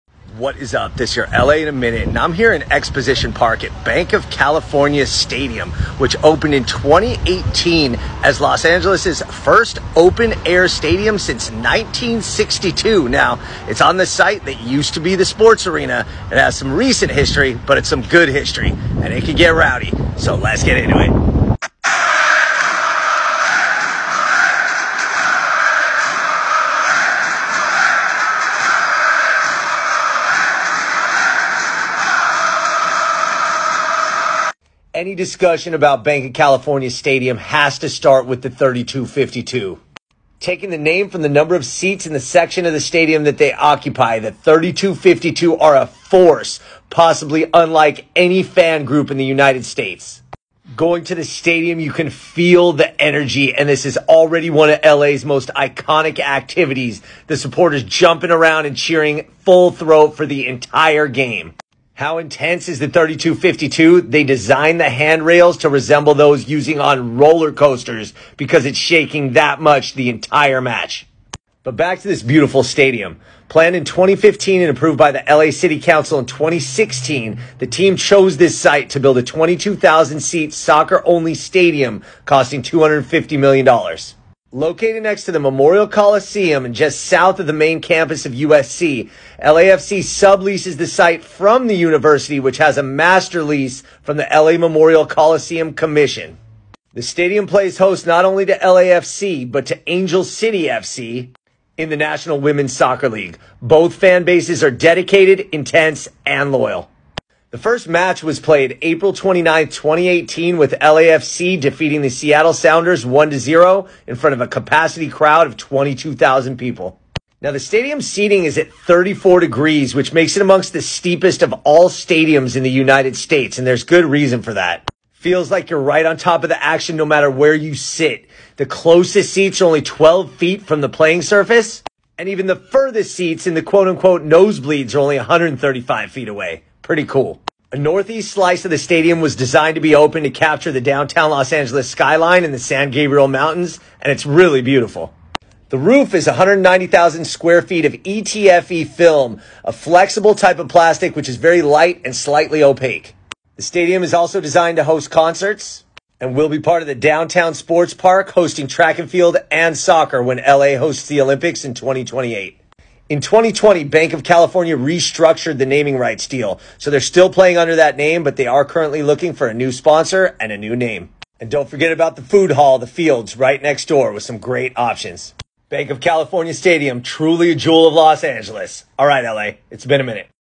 3252 Reasons to love Banc of California Stadium
Banc of California is an intimate stadium in Exposition Park, but it can get rowdy! That’s because of the 3252, what is the most loyal and raucous sections in all of North America. The stadium’s history is short, but it’s awesome. Let’s get into it!